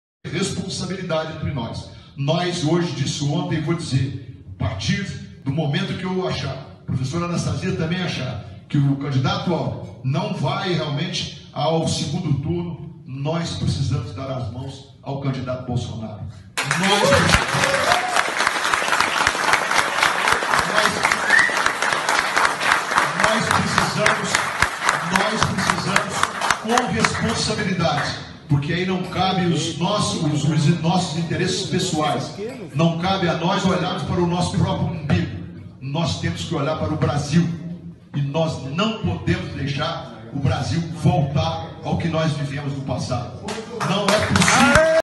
Coligação de Alckmin abandona o barco e apoia Bolsonaro - B17 SHOP
Apoiadores de alckmin já cogitam abandonar a campanha dele para apoiar Jair Bolsonaro.

Compre nossas camisetas e mostre que o DataFolha está errado. 😂 Acesse nossa loja agora mesmo: